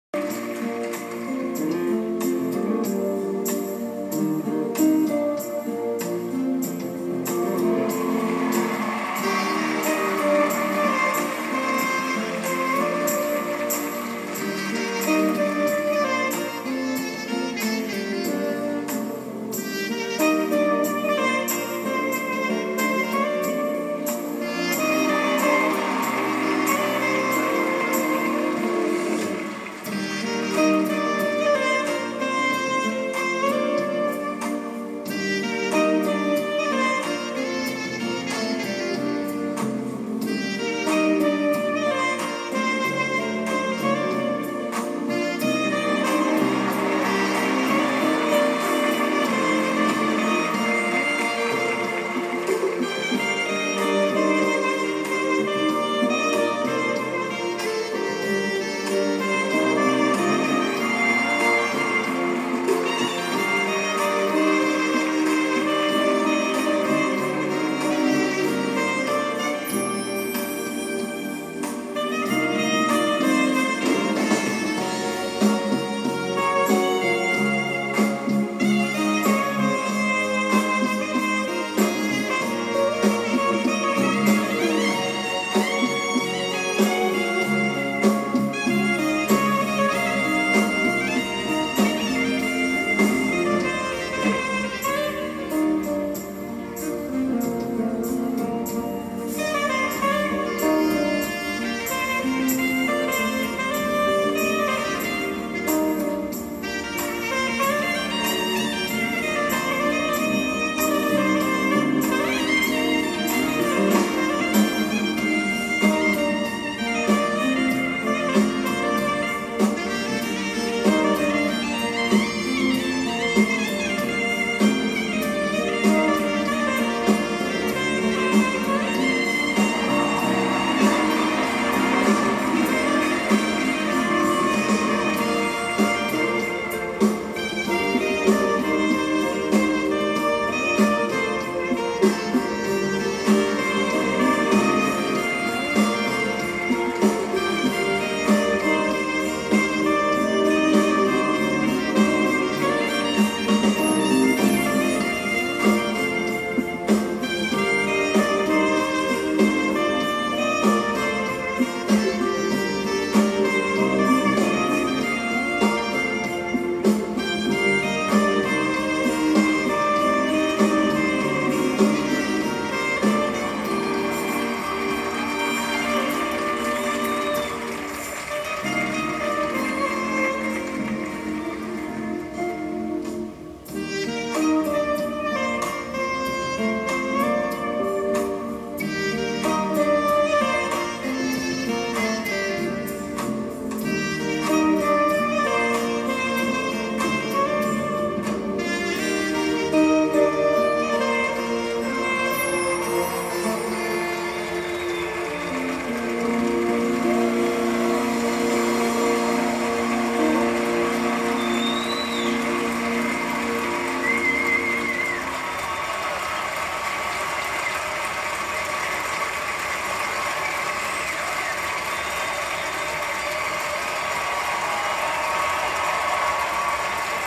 Ilia Kulik with Kenny G
Ilia Kulik skating to Kenny G live in Unforgettable Moments of Love on Ice. // Disson Skating produced for Pandora Skating Series, Jan 26 2012, broadcast Feb 4 2012.